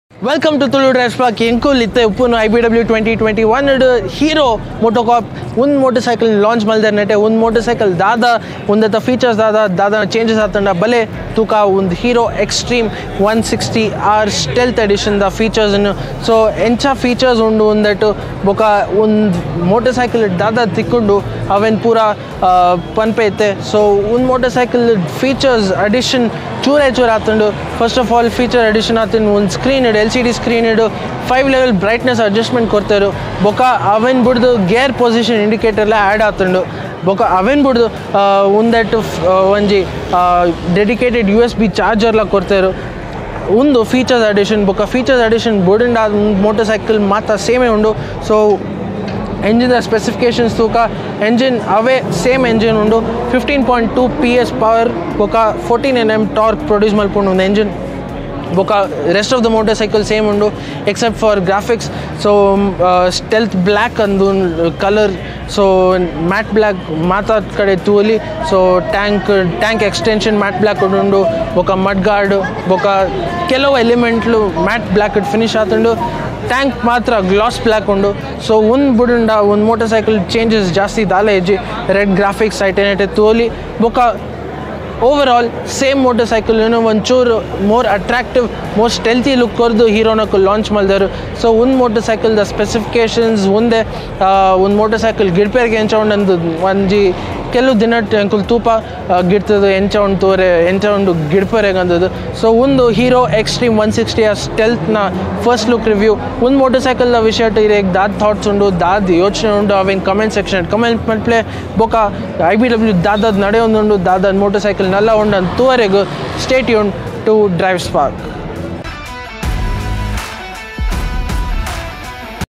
IBW 2021: Hero Xtreme 160R Stealth Tulu Walkaround | Black Paint, LCD Display, 14BHP & More
Hero showcased the recently launched Xtreme 160R Stealth at the 2021 IBW. The motorcycle was showcased with other accessories and apparel at the India Bike Week. Watch the video to find out more.